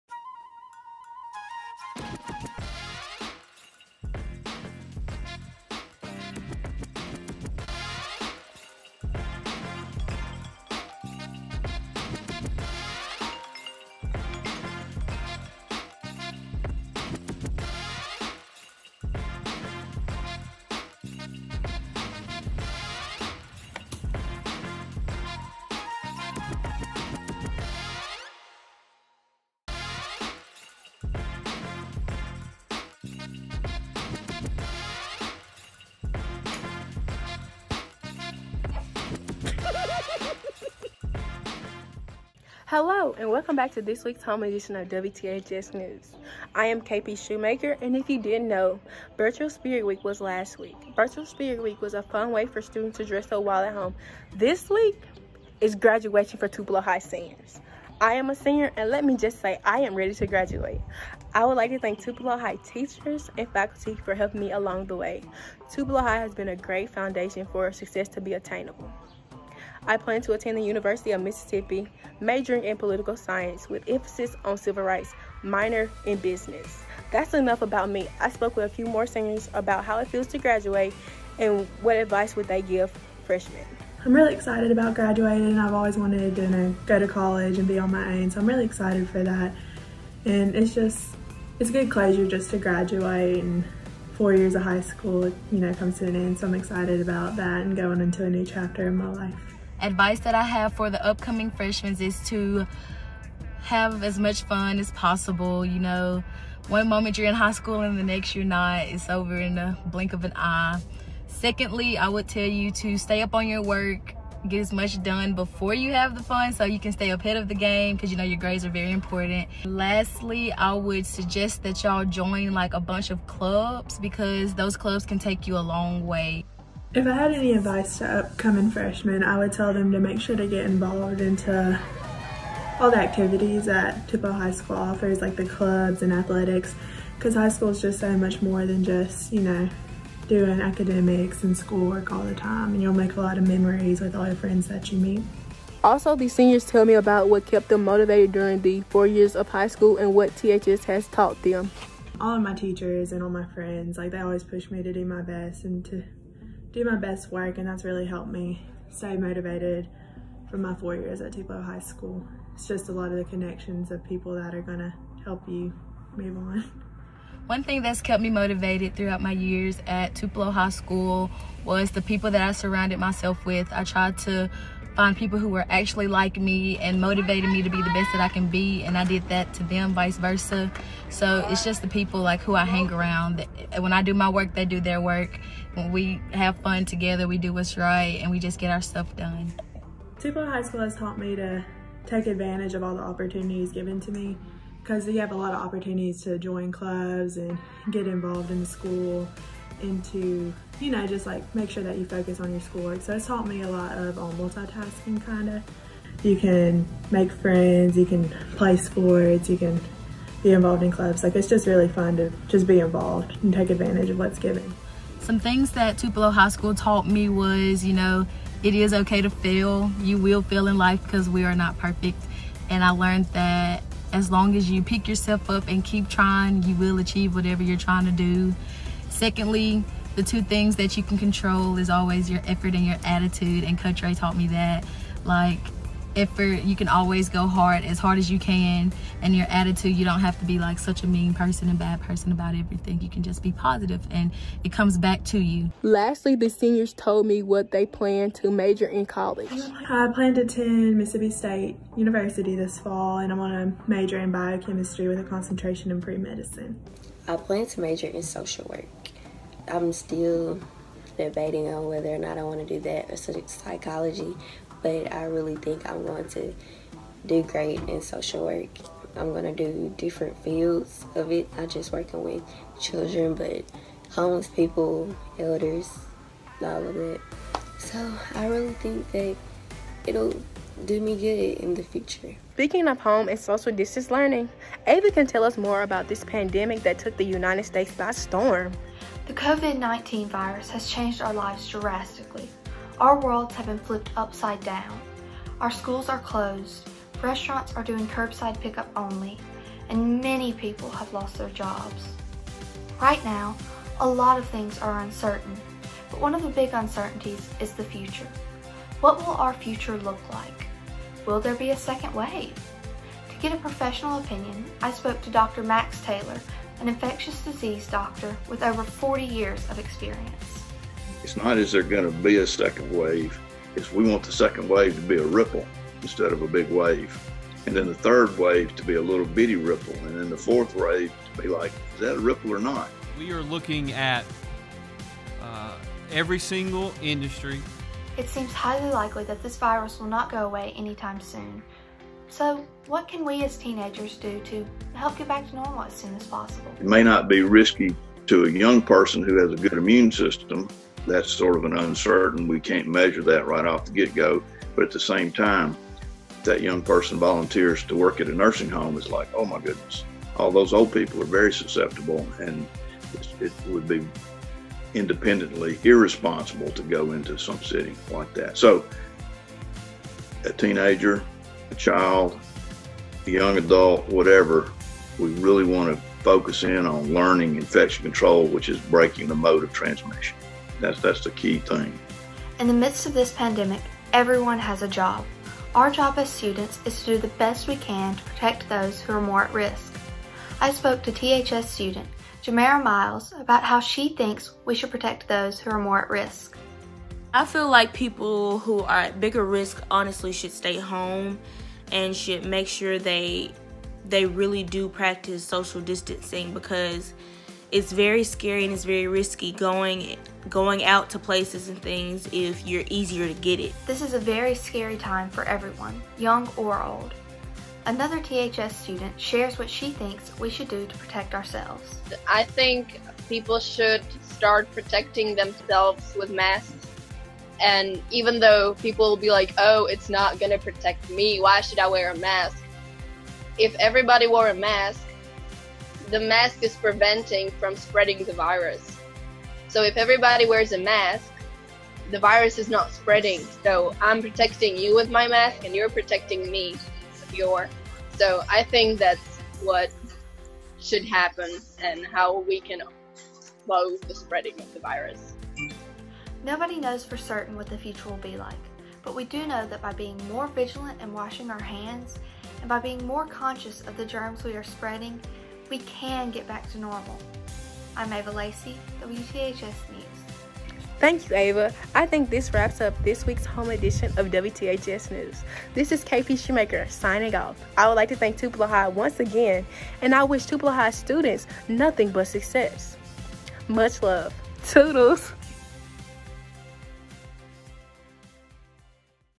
WTHS - "At Home" Edition, May 8th, 2020
THS just wrapped up three days of graduations for more than 450 seniors. KP Shoemaker anchors her final WTHS broadcast. She talks with a couple of seniors about what's next for them after high school and what advice would they give to the freshmen. Ava Lacey discusses with an infectious disease doctor about what could be next when it comes to this pandemic.